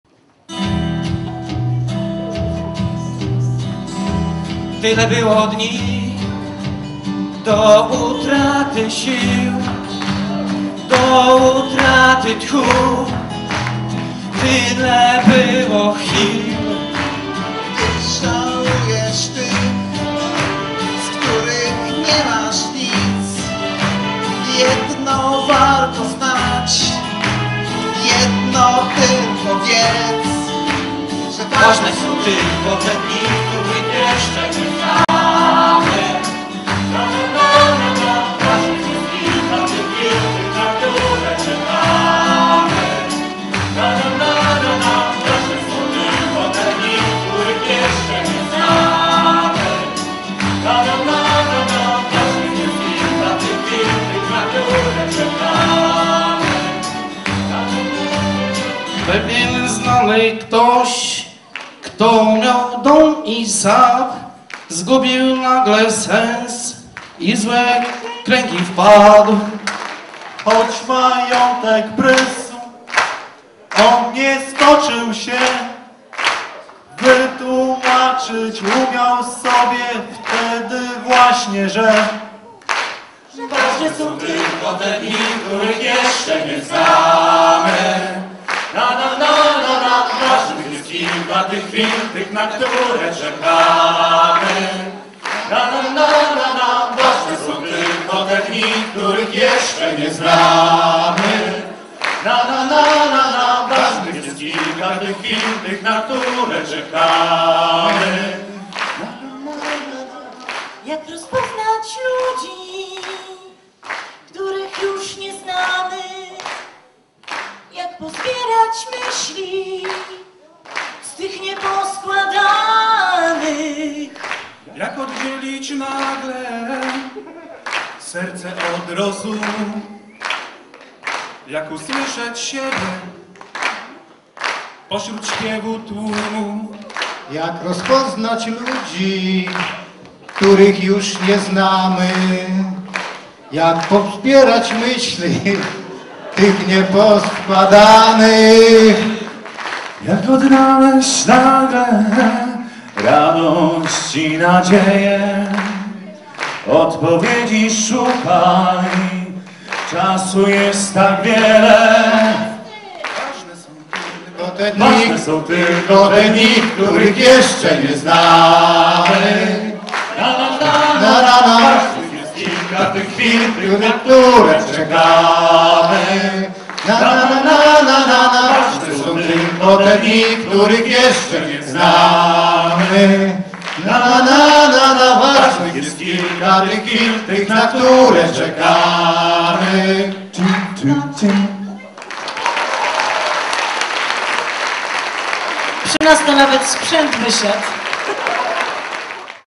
President Band Charytatywnie - bez komentarza
Pełen zaangażowania występ zespołu President Band na koncercie charytatywnym Stowarzyszenia Onkoludki. Pomagającego dzieciom chorym na raka.